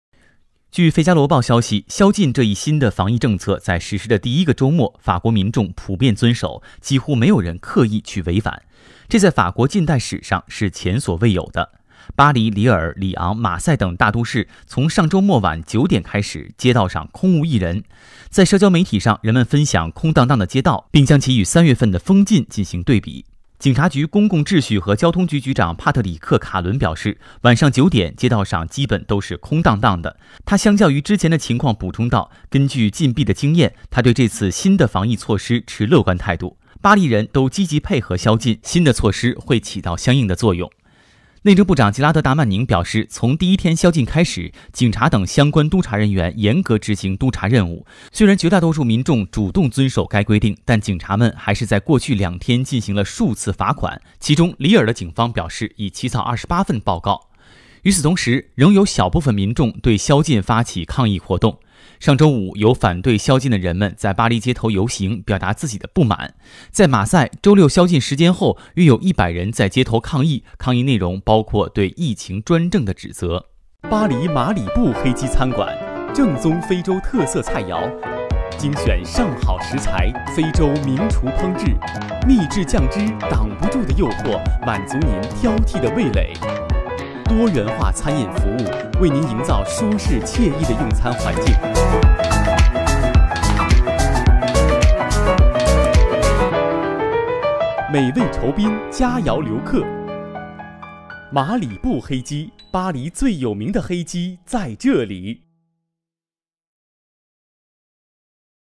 法国民众普遍接受并遵守宵禁
据《费加罗报》消息，这一新的防疫政策在实行的第一个周末，法国民众普遍遵守，几乎没有人刻意去违反，这在法国近代史上是前所未有的：巴黎、里尔、里昂、马赛等大都市本周末从晚上9点开始，街道“空无一人”。在社交网络上，人们分享空荡荡的街道，并将其与三月份的“封禁”进行对比。警察局公共秩序和交通局局长帕特里克·卡伦（Patrick Caron）表示，晚上9点，街道上基本都是空荡荡的。 他相较于之前的情况补充道，根据禁闭的经验，他对这次新的防疫措施持乐观态度。巴黎人都积极配合‘宵禁’，新的措施会起到相应作用。